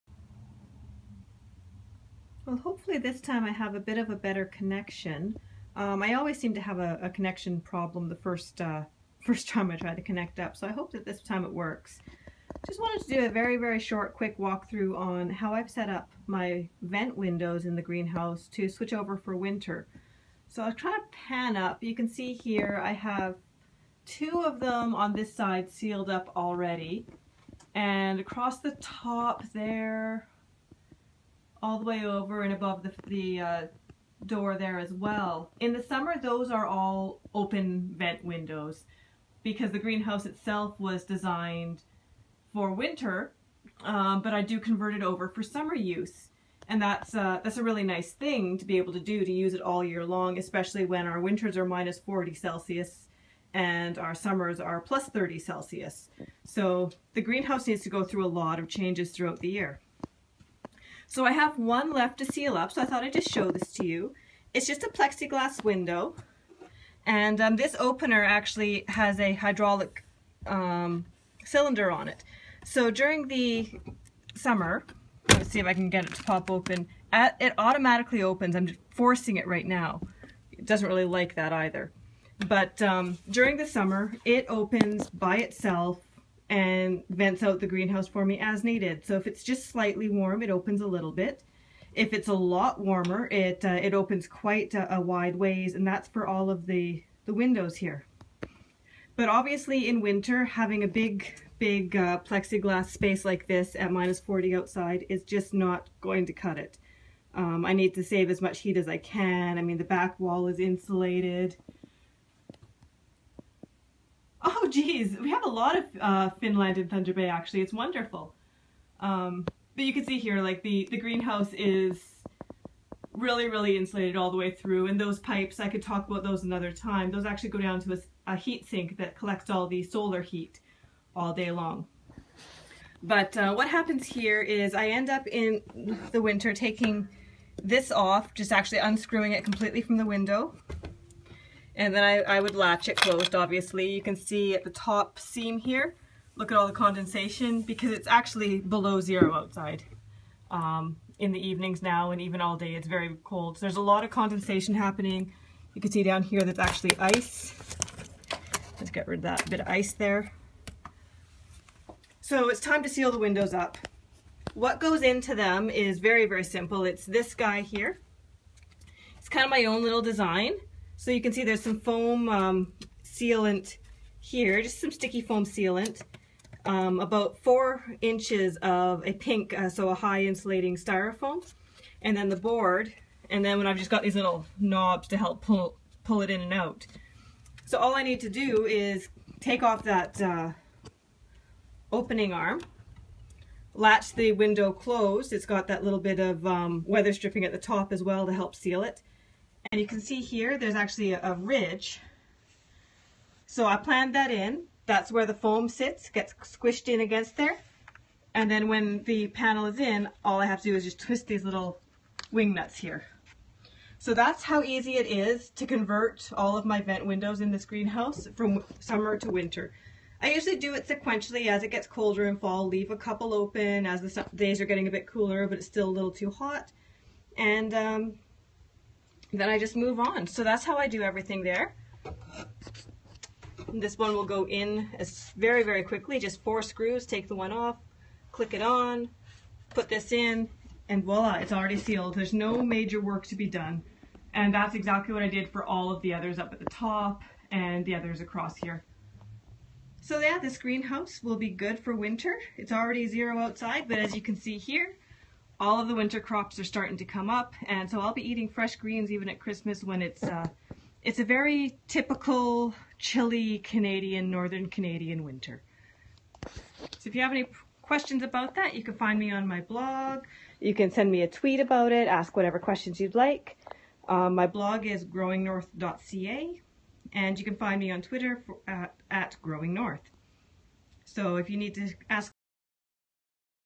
Changing the greenhouse vent windows for winter
Periscope interactive broadcast replay:
The vent windows' ability to be switched from summer to winter states is a main component of my greenhouse.  Here I describe the simple system I built to allow them to go from full vent, to fully insulated in very few steps.
I experiment and grow 365 days a year in the northern boreal climate.  My greenhouse is one of those experimental pieces, allowing me to grow both in winter and in summer, with a small input of resources.